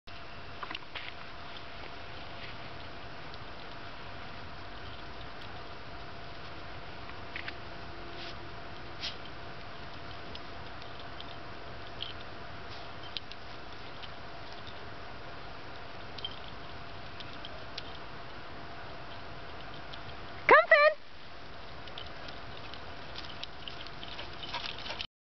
Finn sniffs burrows
Finn sniffing around an open field full of burrows in Tilden park, Berkeley.